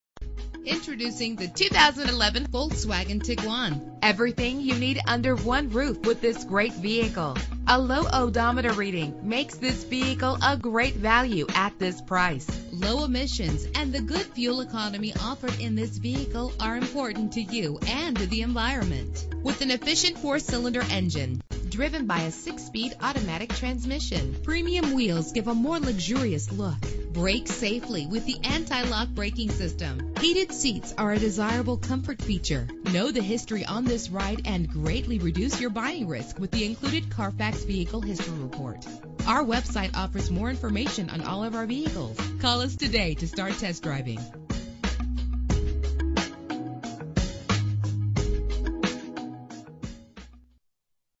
2011 Volkswagen Tiguan - Springfield VA
This vehicle represents similar vehicles available at Volkswagen Springfield, Inc..
Contact Volkswagen Springfield, Inc. at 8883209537, for availability of specific vehicles.
Year: 2011
Make: Volkswagen
Model: Tiguan
Trim: SE
Engine: 2.0 liter inline 4 cylinder 16 valve GDI DOHC Turbo
Transmission: 6-Speed Automatic
Color: Gold
Mileage: 17514
Address:
6601 Backlick Road